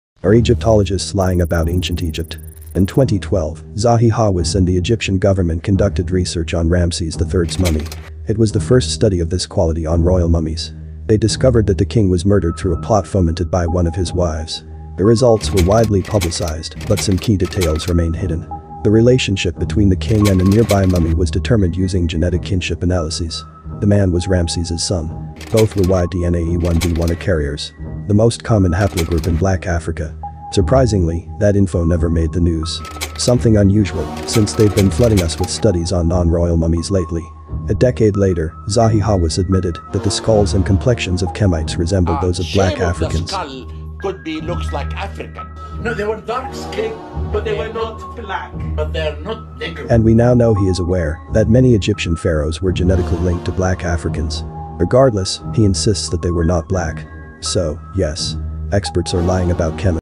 Are Egyptologists Lying About Ancient Egypt?
Zahi knew a decade ago. But he kept denying it.
1. They had facial features similar to Black Africans (skulls/prognathism).
2. They had dark skin. He admitted it himself.
3. Many of them, as well as the pharaohs, were genetically similar to Black Africans (Haplogroup E).
4. They lived in Africa millennia ago.

The question now is, what else do they need to accept the fact that Black people built Kemet?
What makes people Black ?

To me, the most traditional answer is people's facial features and skin color. That is one of the concepts invented by the West to identify those with a lot of melanin in their skin, and those with tropical features (specific shape of skull).

It has no negative connotations, it is a fact. A bit like when ancient Greeks called Africans Ethiopians. It had no negative connotation. It was just a description of what these people they met looked like. We are all humans, but we don't all look the same. Acting as if these minor differences do not exist does more harm than good. We must accept, acknowledge and celebrate it. It's not an issue. When we act like it did not exist we can create more harm like in the case of representation. More and more people are understanding the importance of representation today, especially for people of African descent. Everything has been whitewashed and the African image has been destroyed. That's why we are rebranding it by bringing back the truth.

We are unable to see people's genetics. We make decisions based on appearances. According to these "rules," the ancient Egyptians were Black. When we factor in genetics and location, it becomes undeniable. Nonetheless, specialists still deny this.

This is Black history, and we must be involved. It should be forbidden to talk about ancient Egypt without clarifying the fact that it is started as a Black civilization.

If you are interested in the art appearing in my videos
or on Amazon

1.

2.